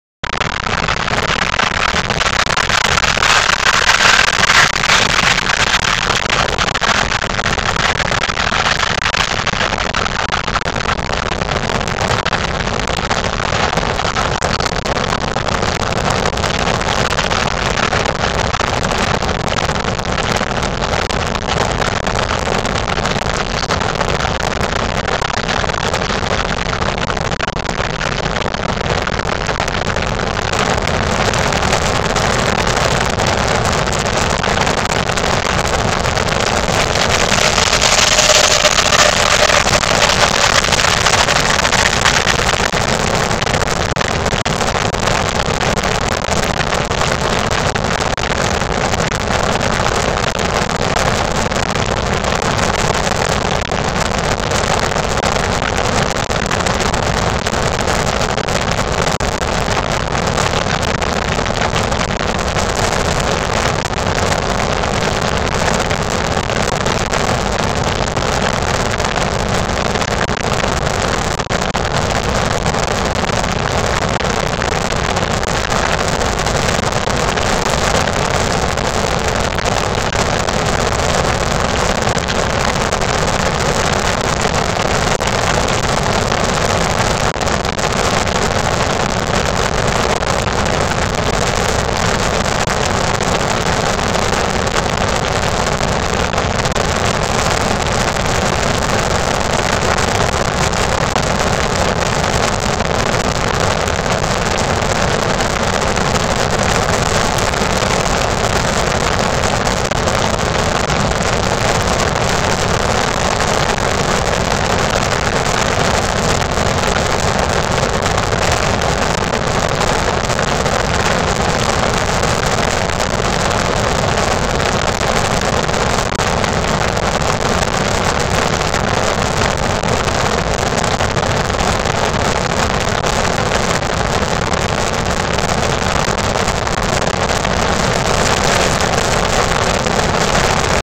Trains and Wyoming
The sprawling Wyoming landscape and the longest train I've ever seen.